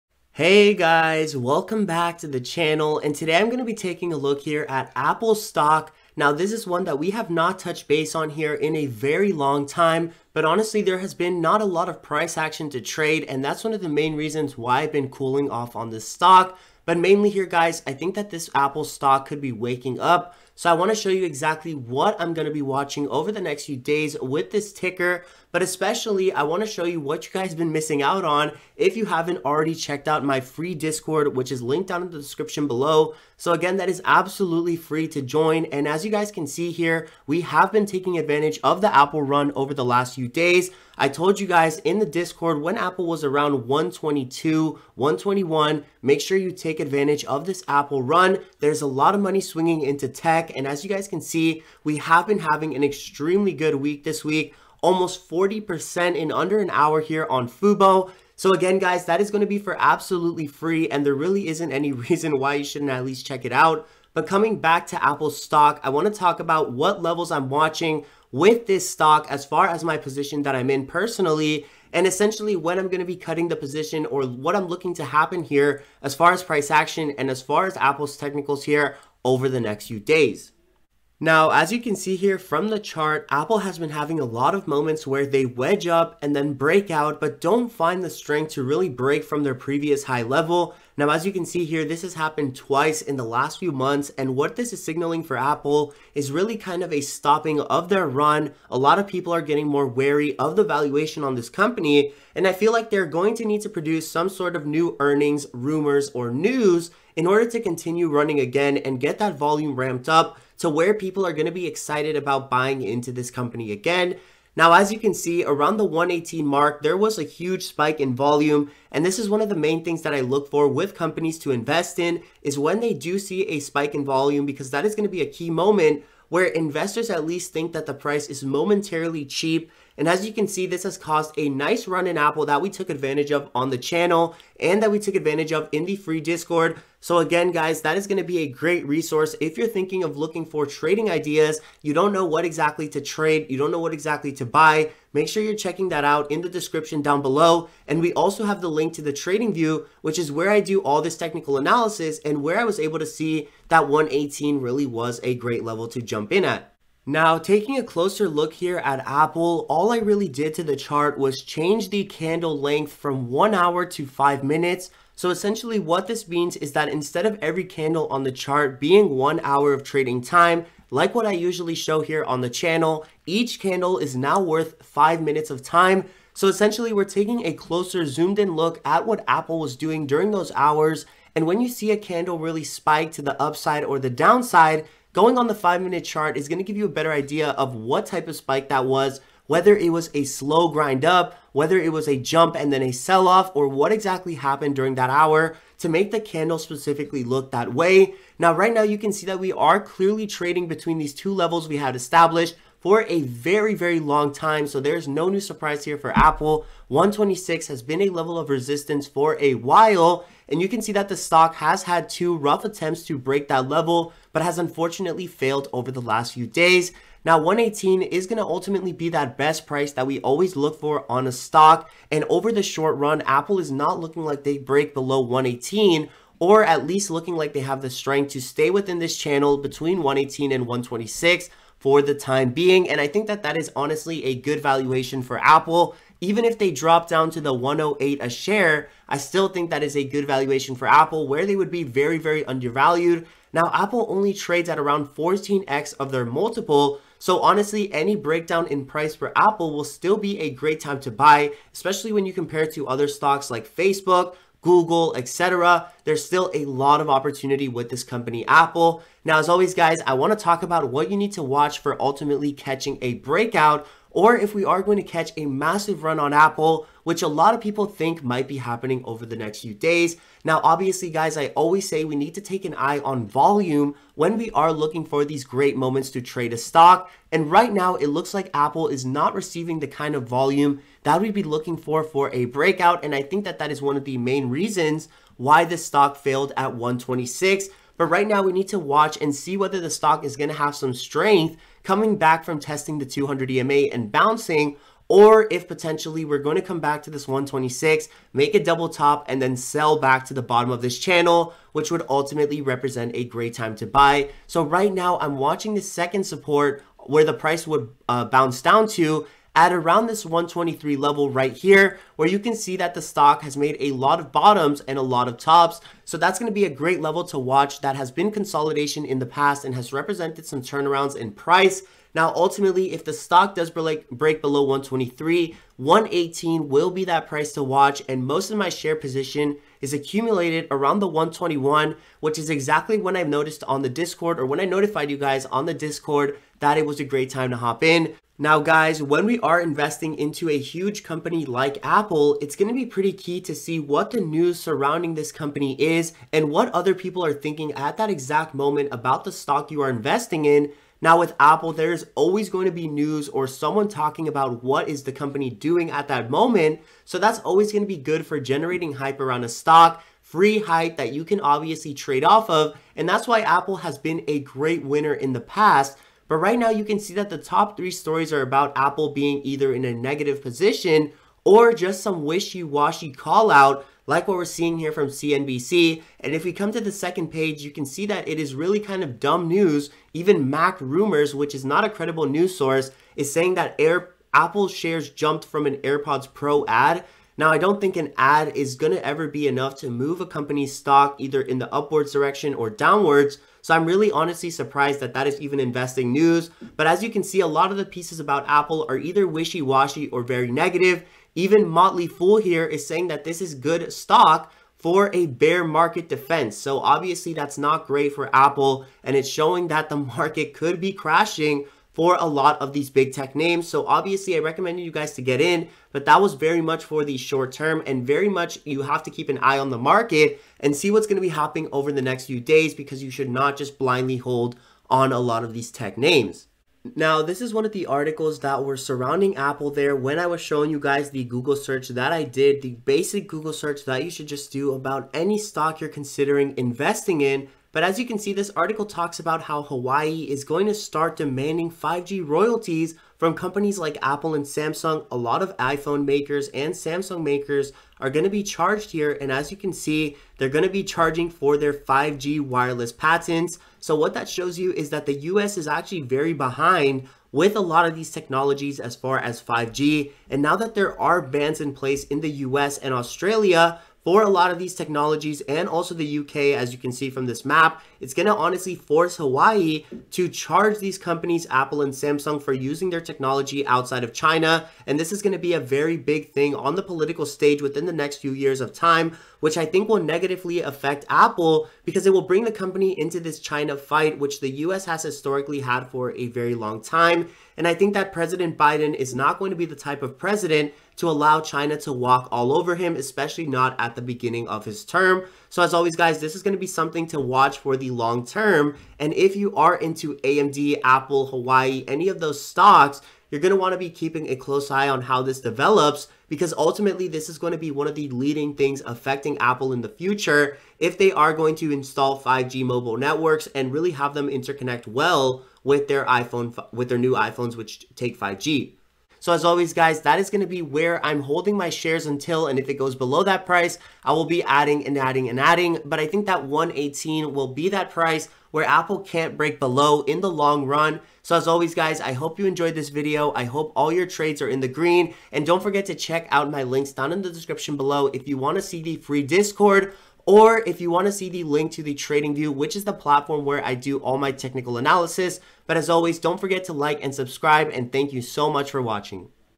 Should You Buy Or Short APPLE Stock @$125? (BREAKOUT ANALYSIS + News) - Robinhood Investing
Want a free stock? Join Robinhood today!

Got plays you're watching? Put them down in the comments below!